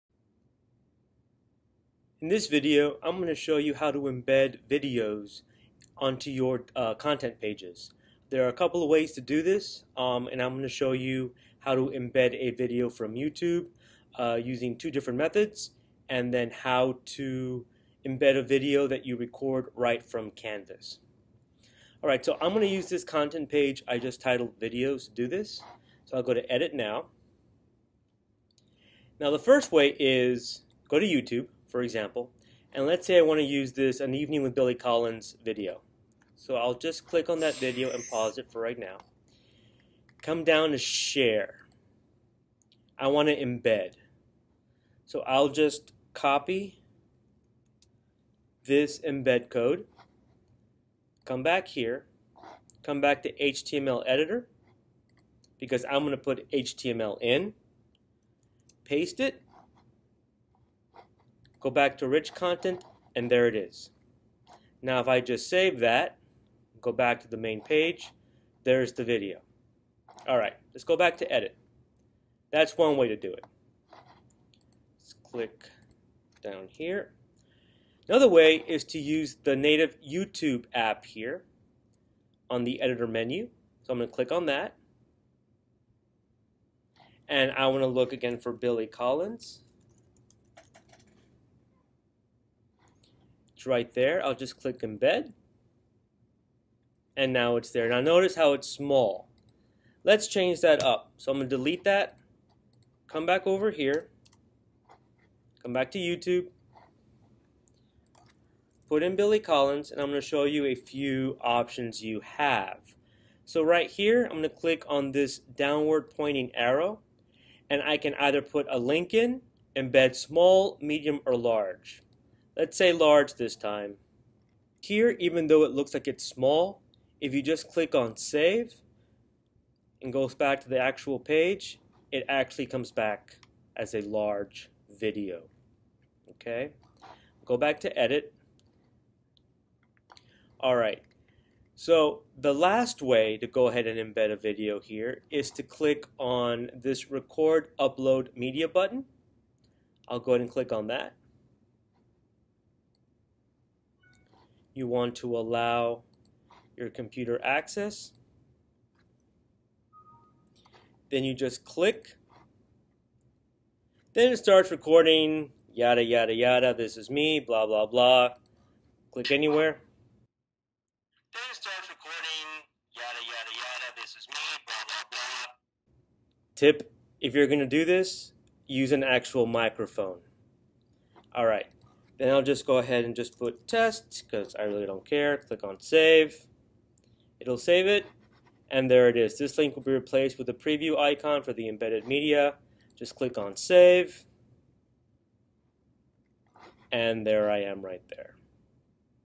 Canvas - Embedding Videos
This video will show you how to embed videos on your content pages.